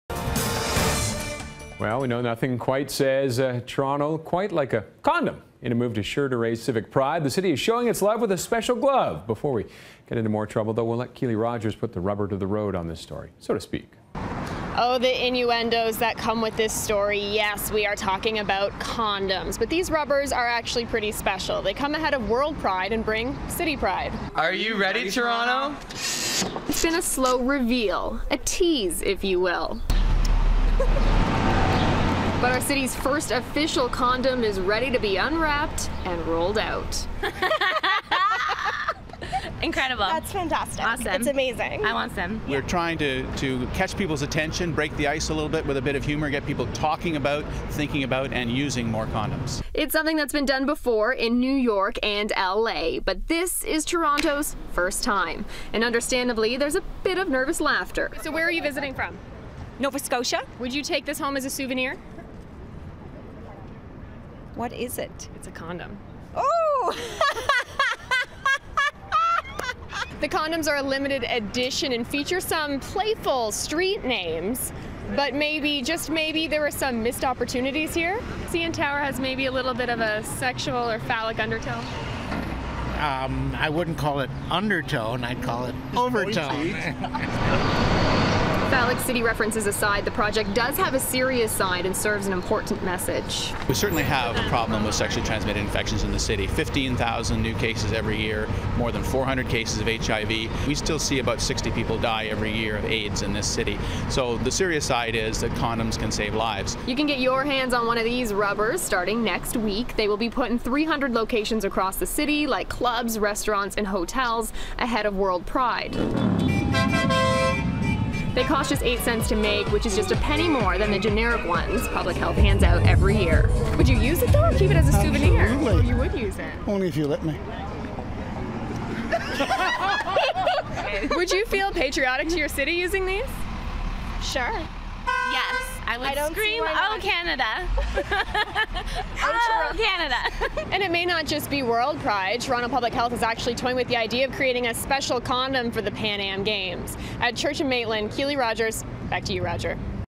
CondomTO rolls out Monday
Toronto's public health department has launched a city-branded condom called condomTO that will be available starting Monday at bars and other locations.